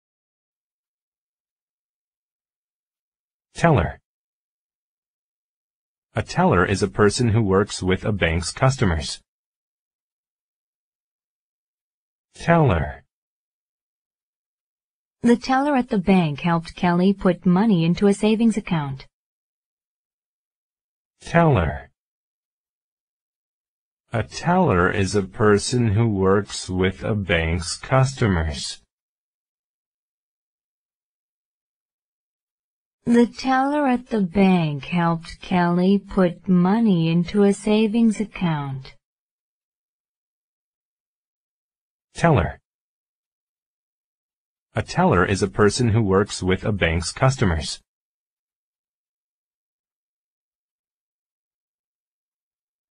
Teller : B2 level english vocabulary lesson
In this english video we will see what the word "Teller" means. It's an English word from B2 level vocabulary list.

The word is "Teller", meaning is: "A teller is a person who works with a bank’s customers." and example is: "The teller at the bank helped Kelly put money into a savings account.".